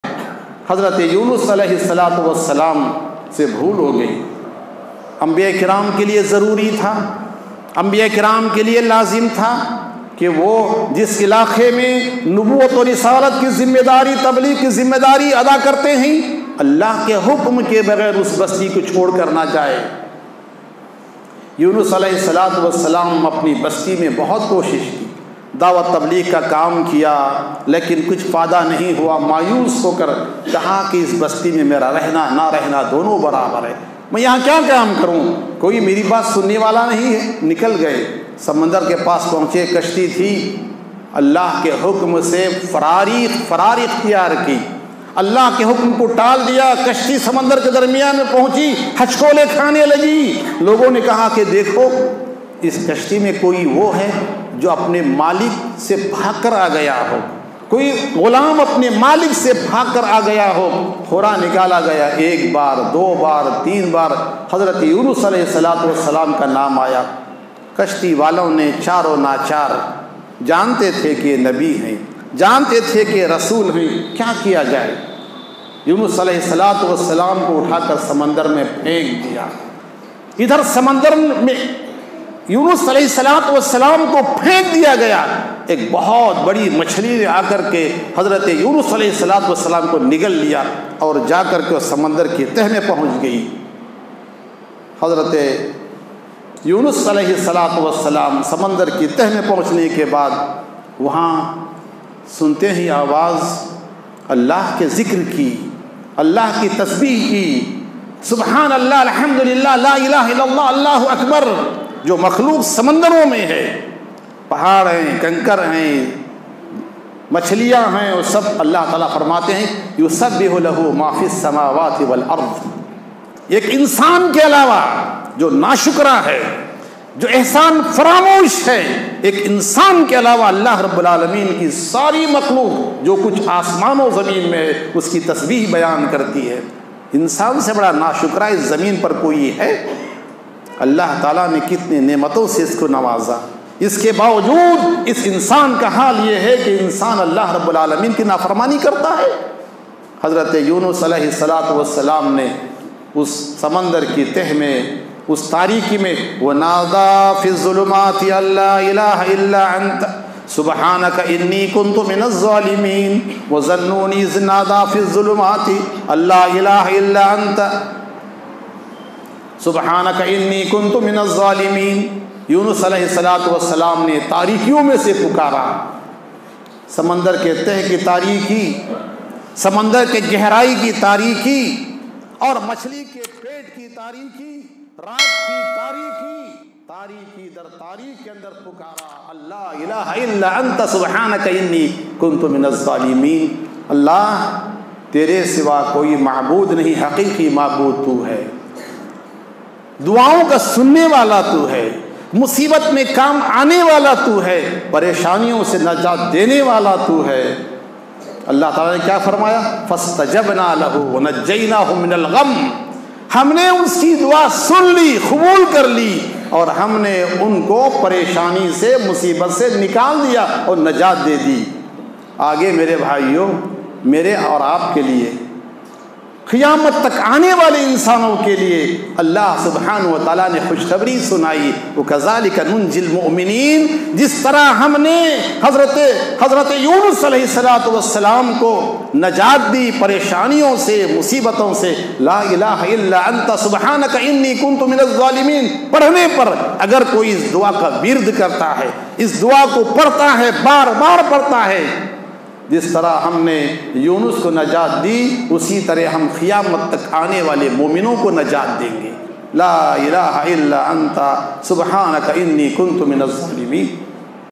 Yunus عليه السلام aur machli ka waqiya | Shaikh Abdul Wahab Abdul Azeez Jamai
This video clips is extracted from Juma'uh khutba _ Duayen kon Sunta Hai_ by Shaikh Abdul Wahab Abdul Azeez Jamai